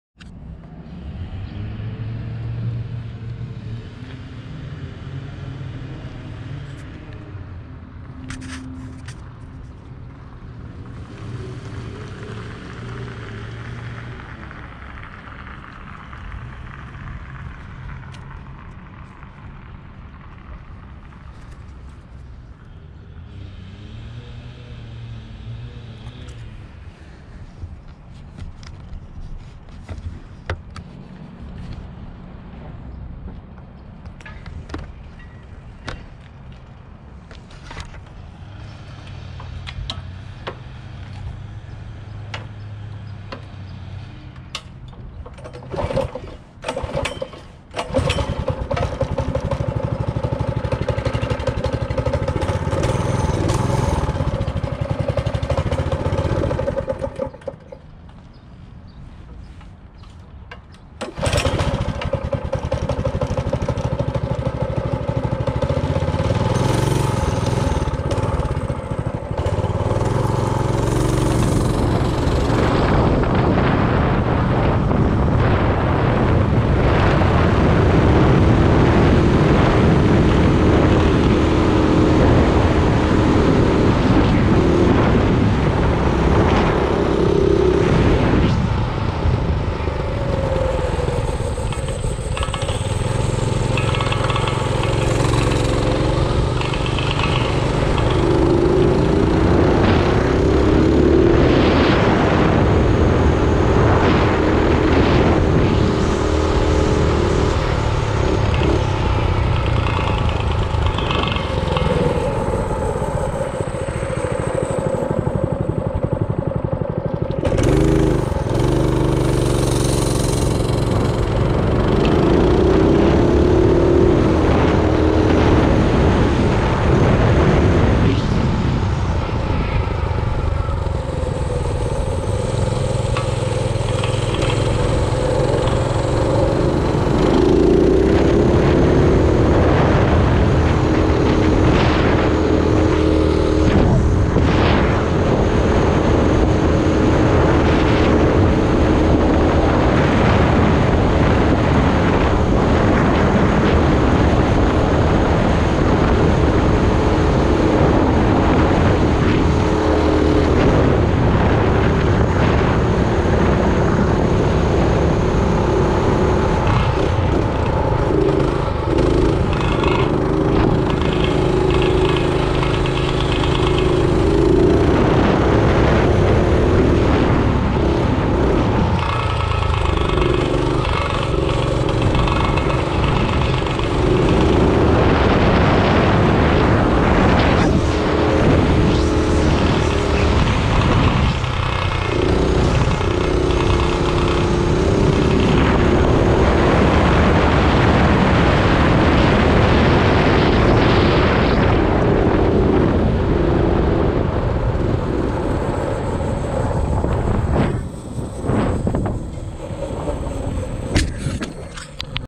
1968 Rupp tt500 mini bike ride
Nice neighborhood cruise, ran out of gas at the end. 1968 Rupp tt500 equipped with a hemi predator 212 and extended swingarm.